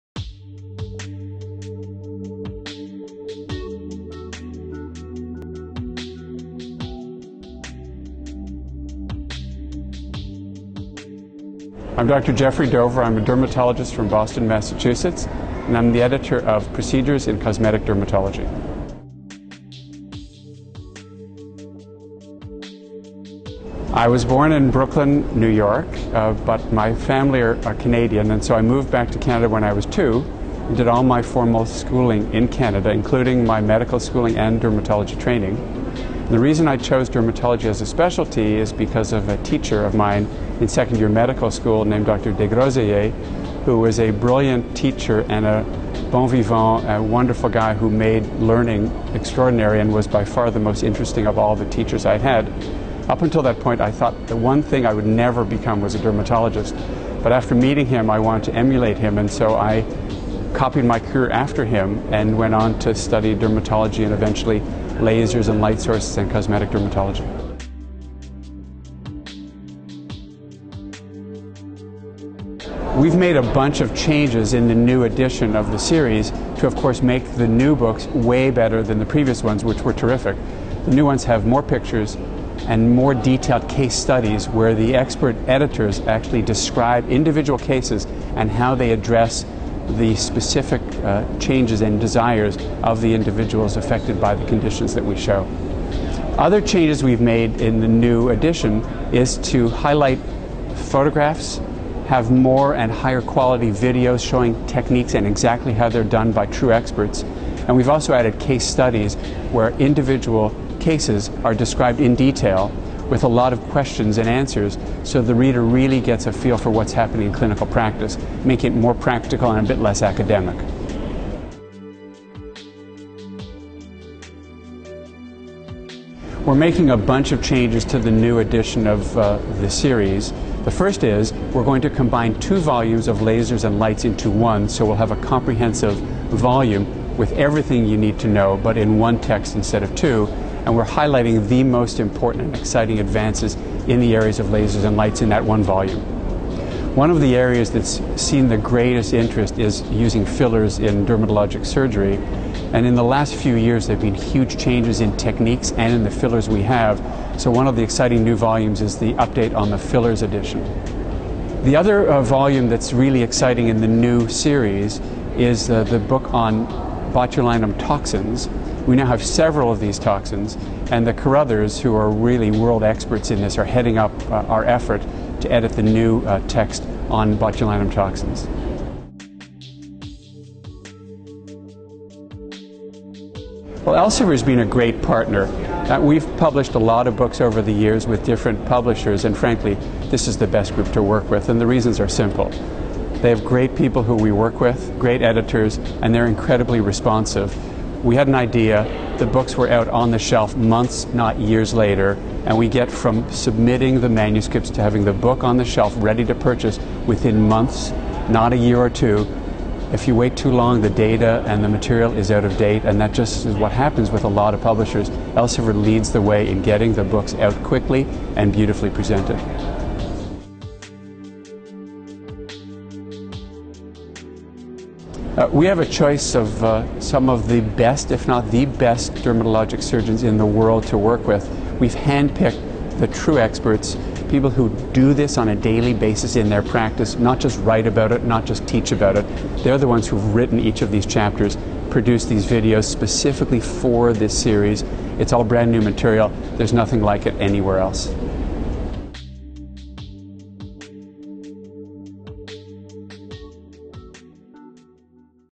Dr. Jeffrey Dover, an Elsevier Author, at AAD 2012
Dr. Dover explains the new additions to the "Procedures in Cosmetic Dermatology Series," including more photos, detailed case studies, a consolidated volume on lights and lasers, updates on fillers and botulinum toxins, and the hand-picked experts selected to write its chapters.

To learn more about Dr. Dover, please visit his official Elsevier Authors